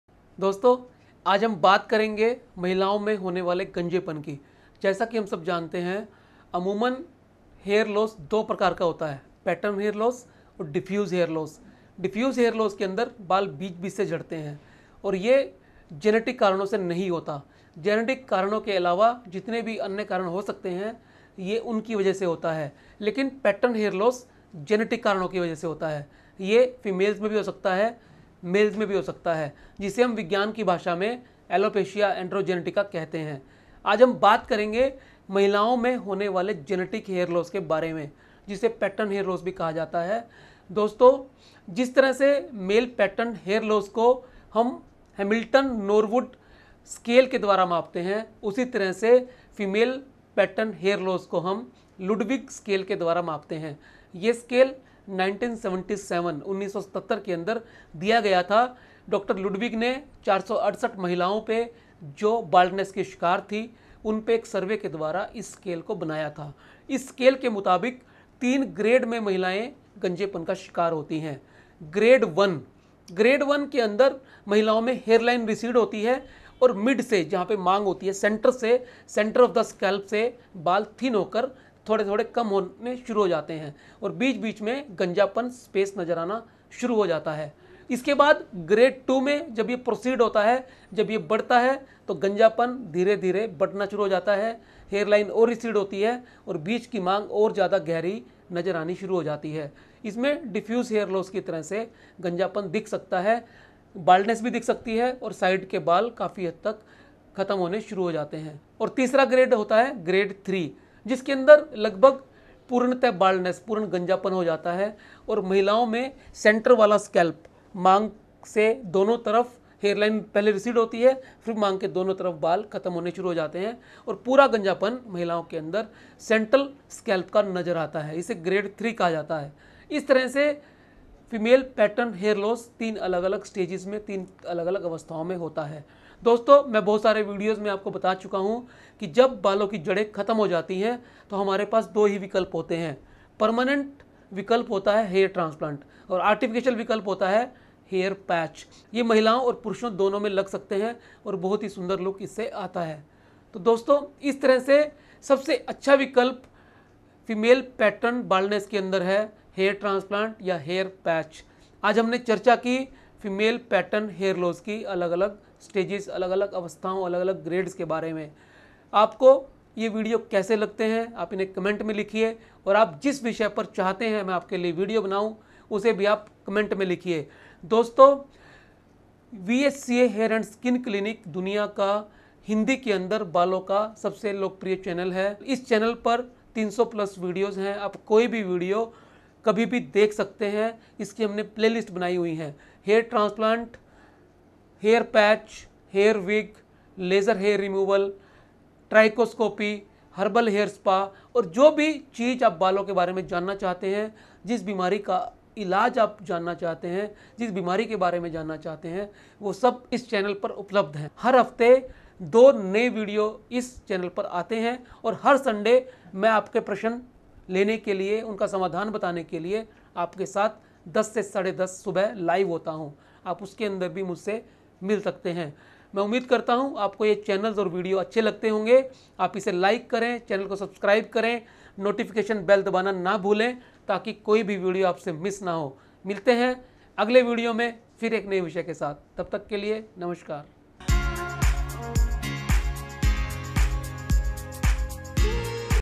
महिलाओं में गंजेपन की 3 अवस्थाएँ | Female Pattern Hair Loss | VHCA HAIR CLINIC | Delhi and Haryana
महिलाओं में गंजेपन की 3 अवस्थाएँ | Female Pattern Hair Loss | VHCA HAIR AND SKIN CLINIC | Delhi and Haryana

महिला पैटर्न गंजापन जिसे एंड्रोजेनेटिक एलोपेसिया भी कहा जाता है, बालों का झड़ना है जो महिलाओं को प्रभावित करता है। यह पुरुष पैटर्न गंजापन के समान है, सिवाय इसके कि महिलाएं पुरुषों की तुलना में अपने बालों को एक अलग पैटर्न में खो सकती हैं। महिलाओं में बालों का झड़ना सामान्य है, खासकर जब आप उम्र के रूप में। रजोनिवृत्ति के बाद दो-तिहाई महिलाएं बालों के झड़ने का अनुभव करती हैं।

- महिला पैटर्न गंजापन कैसा दिखता है?
महिलाओं में, महिला पैटर्न गंजापन का पहला संकेत एक चौड़ा हिस्सा या ऐसा महसूस हो सकता है कि बाल हमेशा की तरह घने नहीं लगते। हालांकि खोपड़ी दिखाई दे सकती है, हेयरलाइन आमतौर पर नहीं आती है।

महिला पैटर्न गंजापन और उसके उपचार के बारे में जानने के लिए यह वीडियो देखें।
Female pattern baldness also called androgenetic alopecia, is hair loss that affects women. It's similar to male pattern baldness, except that women can lose their hair in a different pattern than men. Hair loss in women is normal, especially as you age. Up to two-thirds of women experience hair loss after menopause.

- What does female pattern baldness look like?
In women, the first signs of female pattern baldness may be a widening part or a feeling that the hair does not feel as thick as usual. Although the scalp may be visible, the hairline usually does not recede.

Watch this video to know about the female pattern baldness and its treatment.